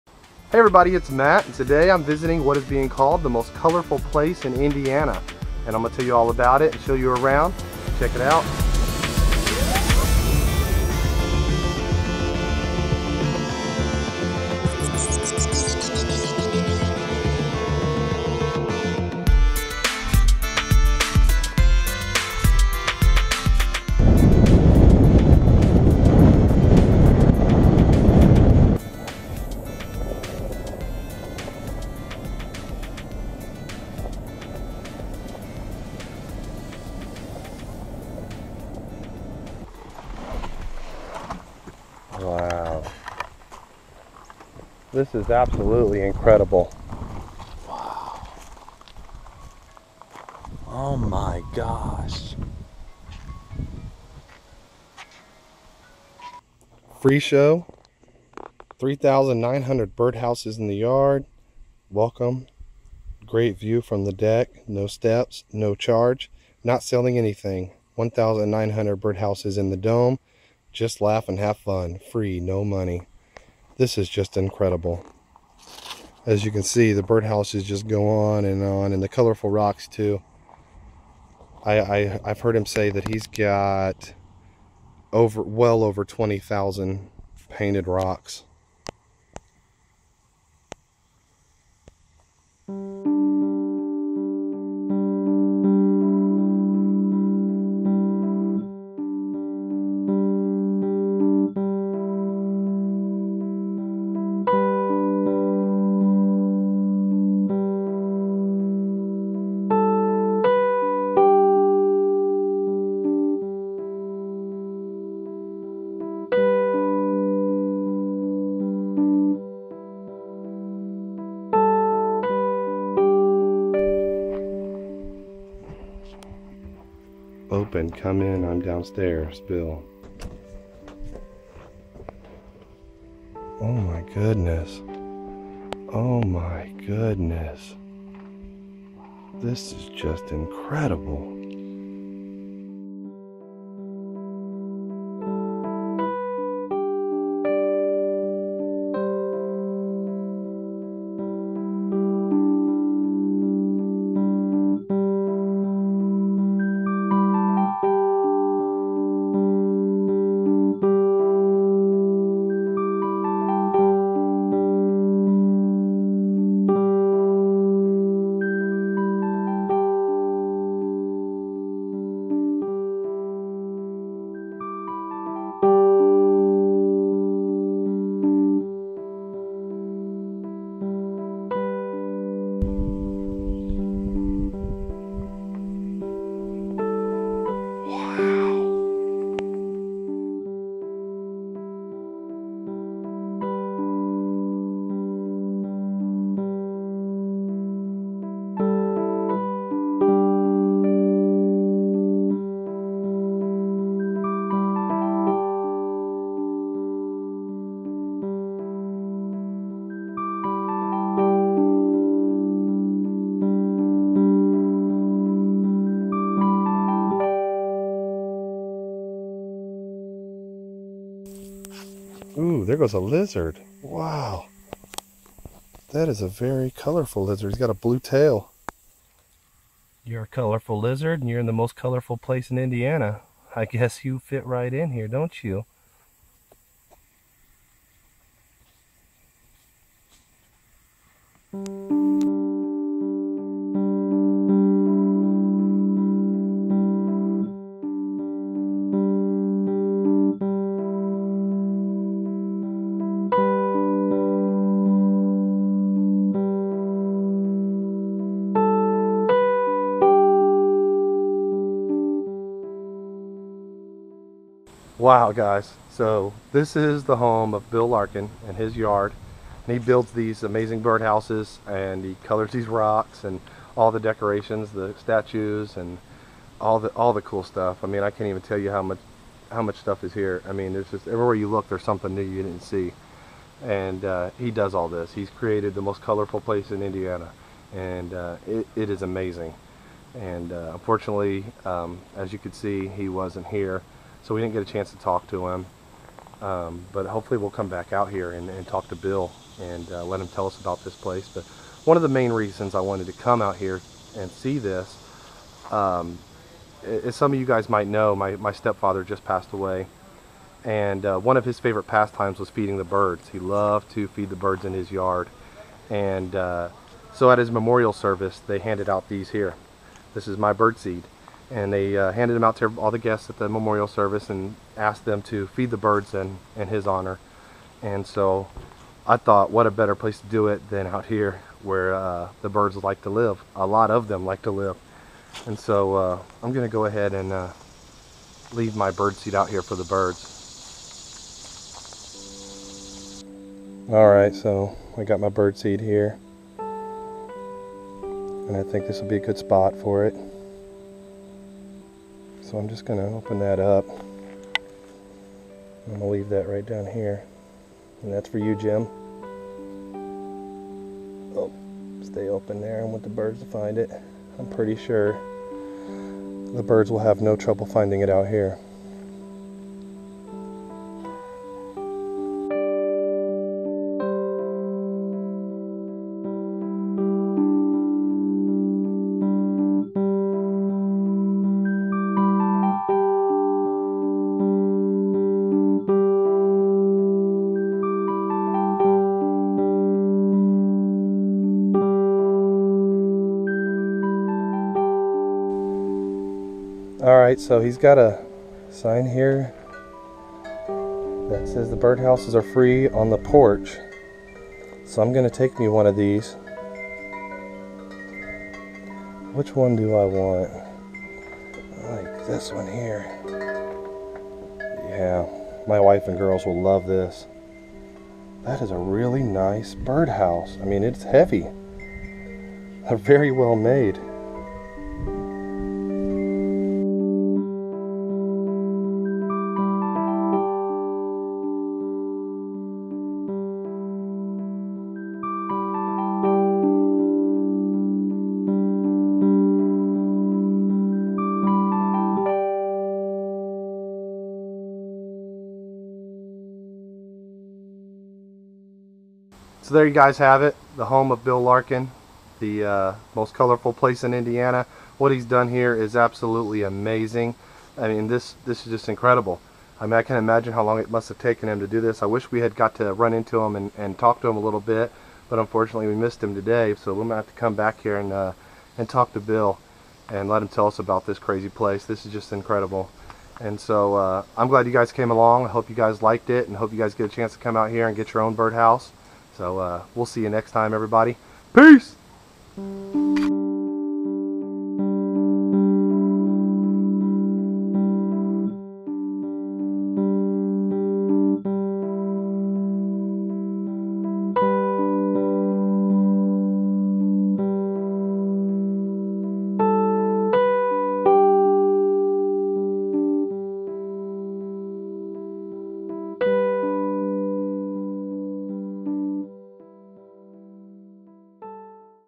The Most Colorful Place in Indiana
In this video, Matt sets out to find a perfect place to feed his birdseed to the birds in honor of his Step-Father, Jim, and what he finds is absolutely astounding! This mind-blowing place is known as the most colorful place in Indiana. We found this place in a very rural Loogootee, Indiana, less than two miles north of St. Mary's Church. Bill Larkin, the owner and creator of this amazing place, builds about 1,000 birdhouses a year, and gives them away for free to visitors! We are very glad we visited this place, and you will too.